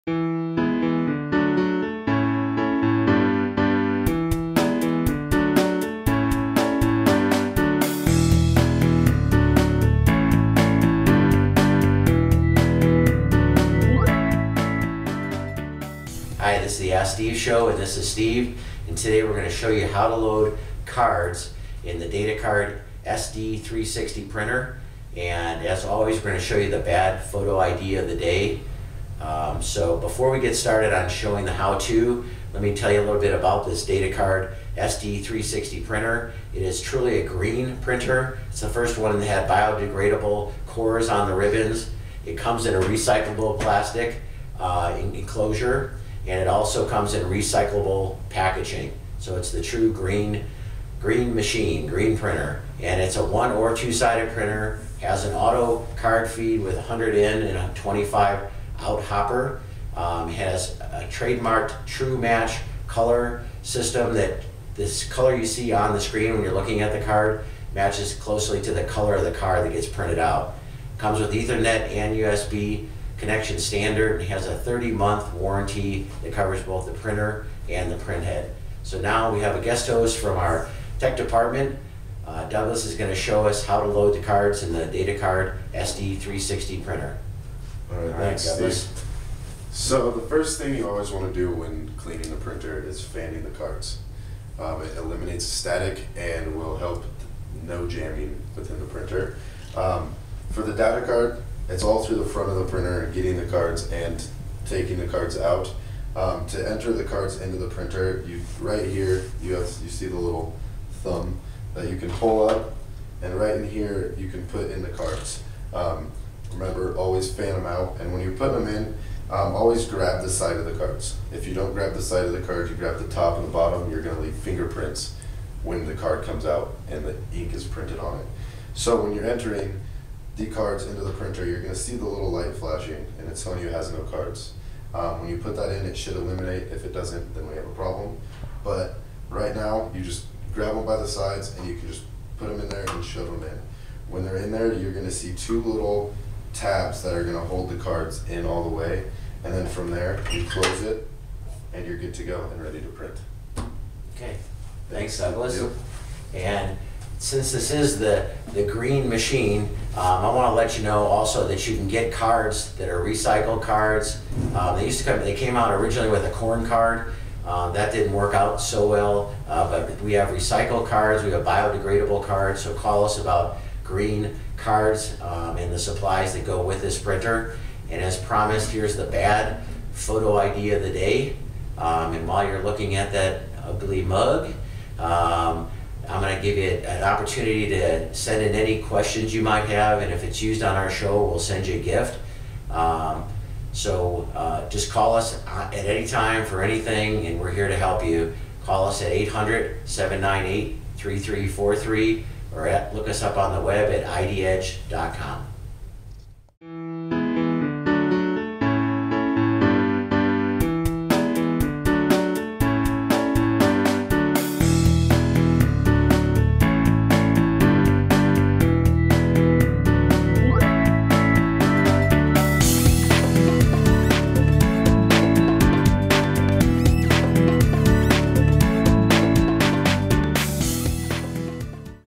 SD360 card printer by Datacard | ID Badges | Colorado Springs Printers
A step by step instructional to loading cards into the Datacard SD360 card printer.